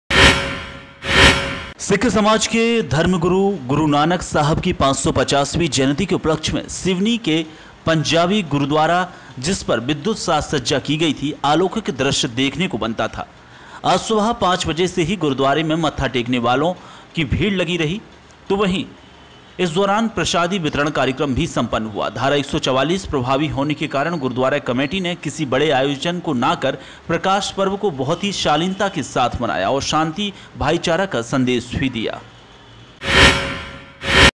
NEWS सिवनी- गुरु नानक साहब की 550वीं जयंती के उपलक्ष में कार्यक्रम आयोजित
सिवनी- गुरु नानक साहब की 550वीं जयंती के उपलक्ष में कार्यक्रम आयोजित